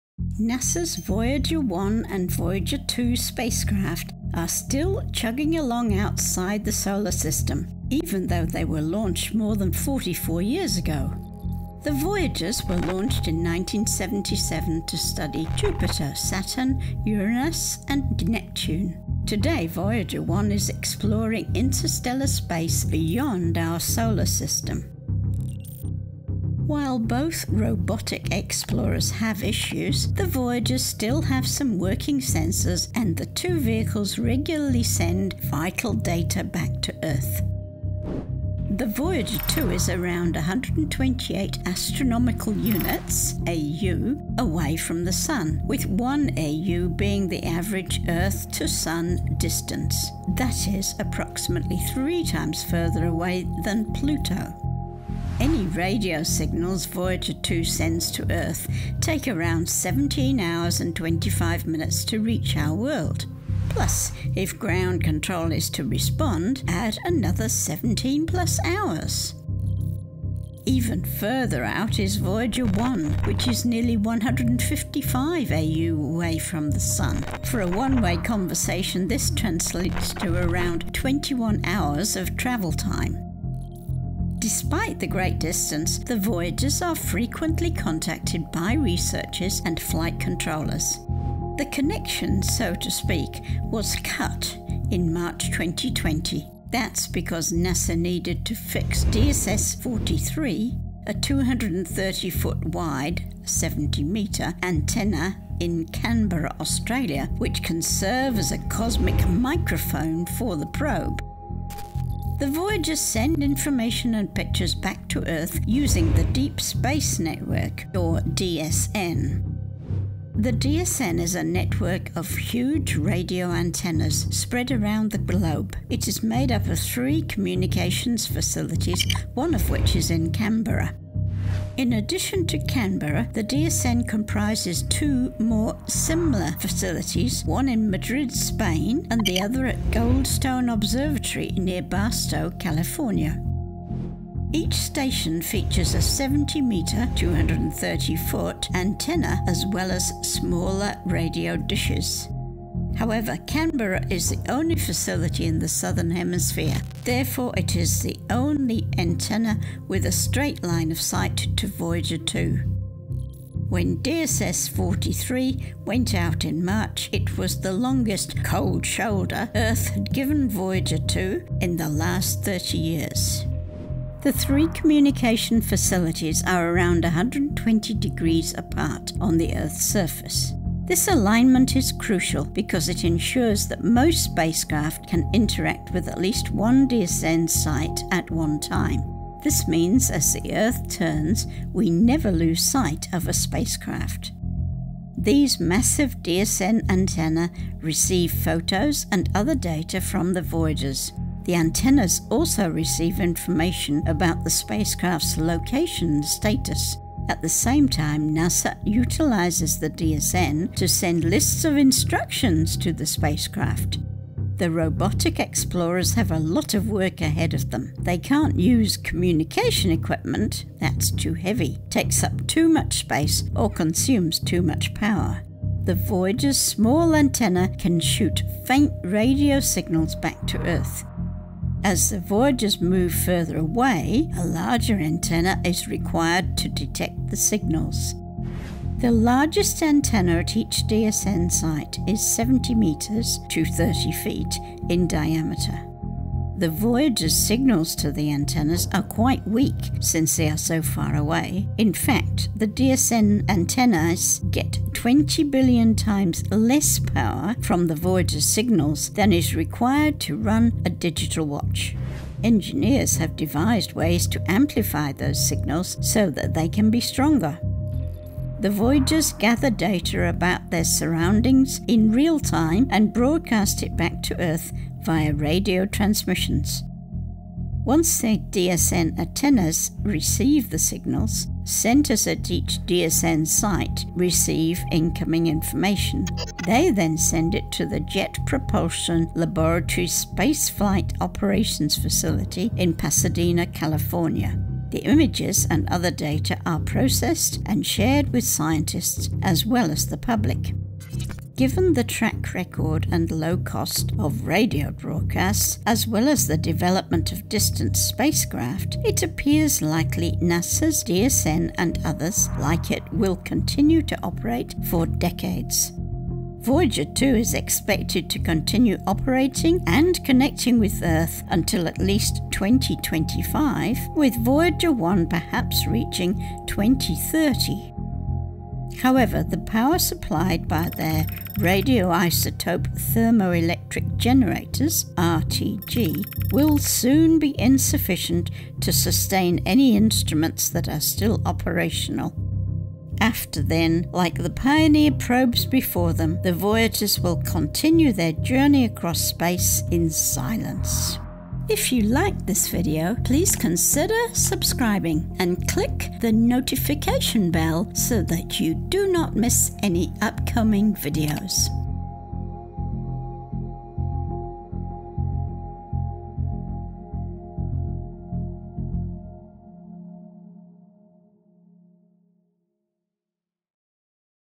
How Did Voyager Spacecraft Send Pictures Back?
NASA's Voyager 1 and Voyager 2 spacecraft are still chugging along outside the solar system, even though they were launched more than 44 years ago. But how do scientists communicate with them?

Produced & Edited by:
Ardit Bicaj

Written by:
Nicole Amondi

Narrated by:
Janice M. Baird

Graphics:
Universe Sandbox 2
Space Engine
iStock.com/Mediablix
NASA's Scientific Visualization Studio
NASA
Goddard Space Flight Center Conceptual Image Lab
NASA's Eyes